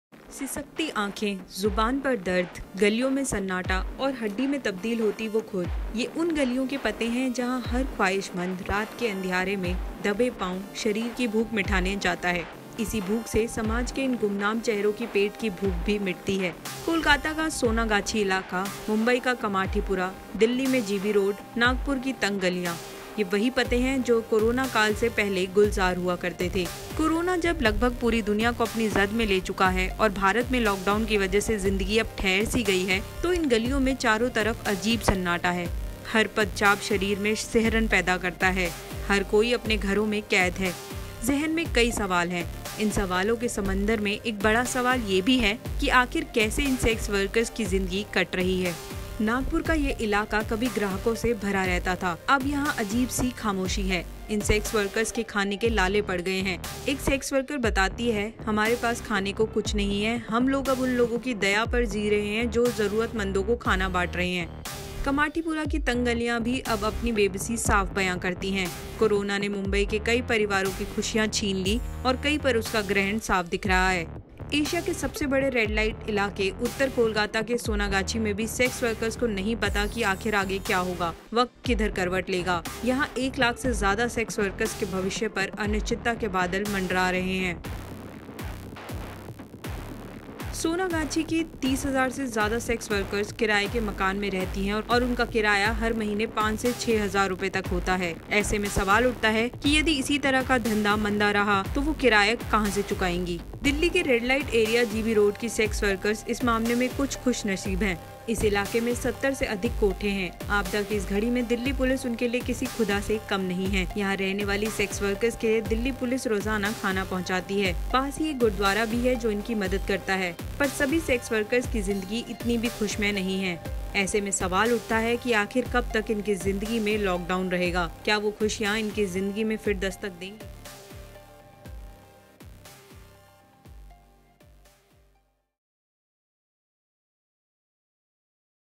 Lockdown 2: Gb Road Sonagachi की Sex Workers की जिंदगी कैसे बदल दी Coronavirus ने
Gb Road Sonagachi Sex Workers Life In Coronavirus Lockdown
कोरोना वायरस (Coronavirus) के बाद लॉकडाउन (lockdown) ने सेक्स वर्कर्स (sex workers) से बहुत कुछ छीन लिया है। कभी उनकी गलियां गुलजार रहा करा करती थीं, जहां अब सन्नाटा है। लॉकडाउन ने एक तरह से उनकी जिंदगी को लॉक सा कर दिया है।